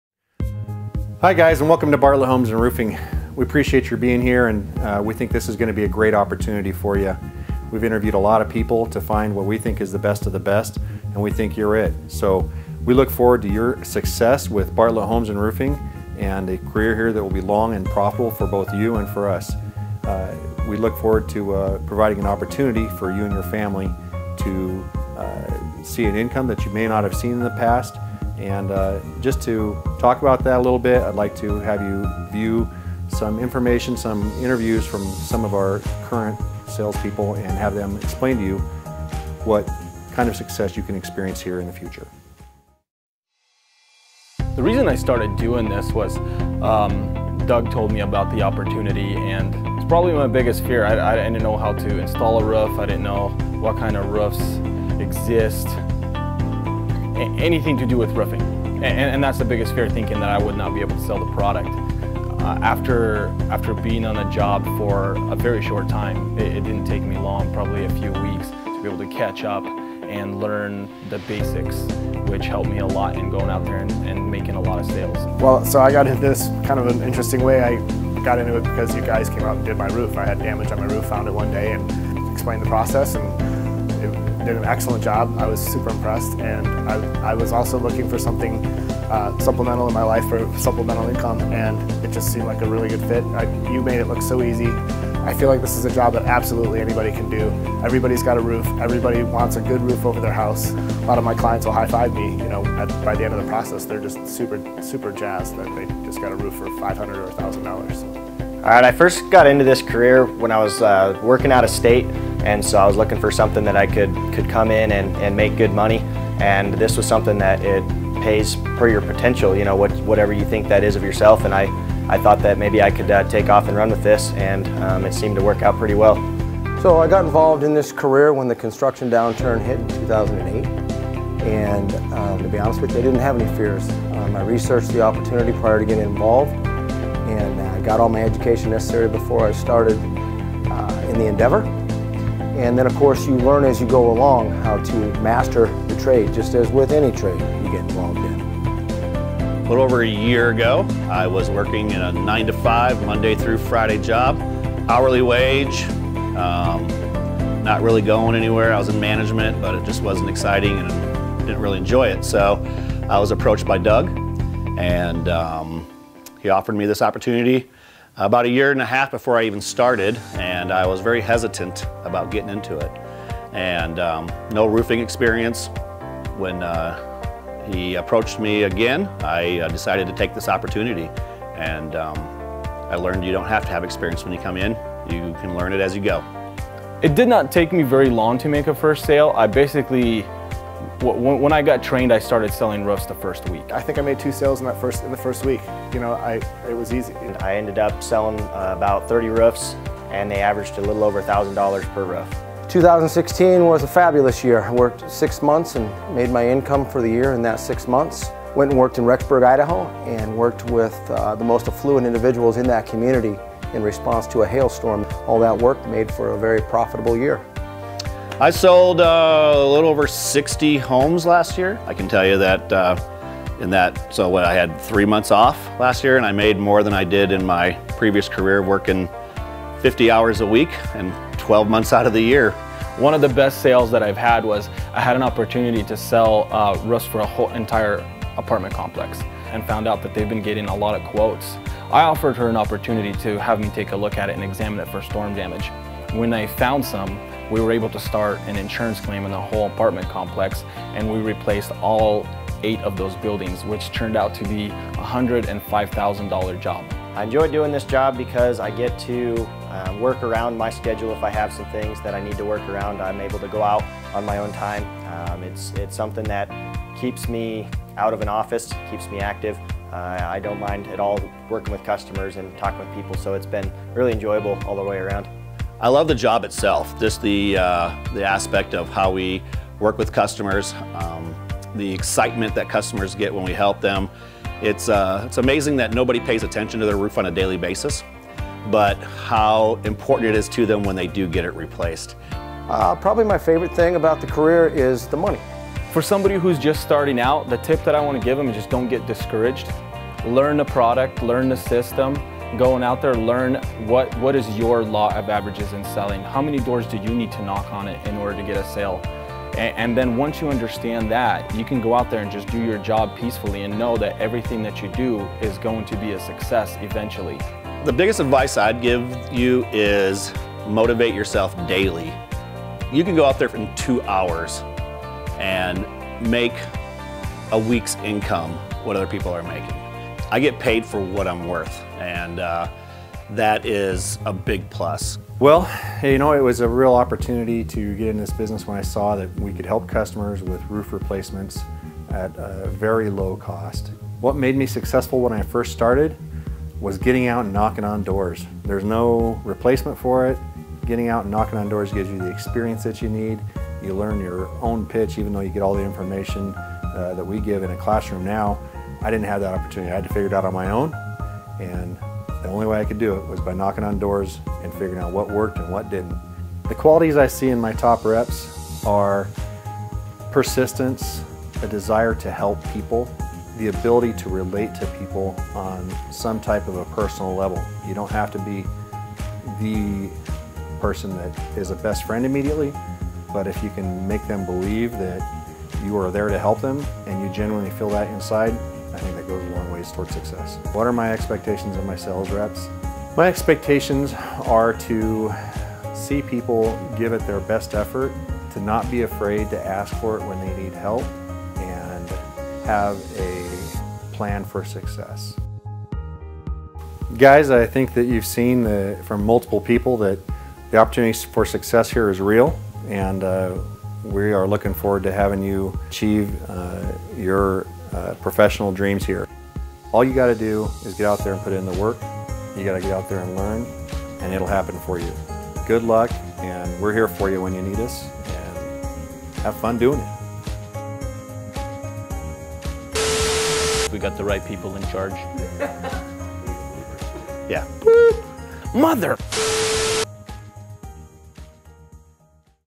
Bartlett Roofing - Sales Training
Here at Bartlett Homes we've decided to interview our salesman and sales manager about their experience and advice on becoming a roofing saleman.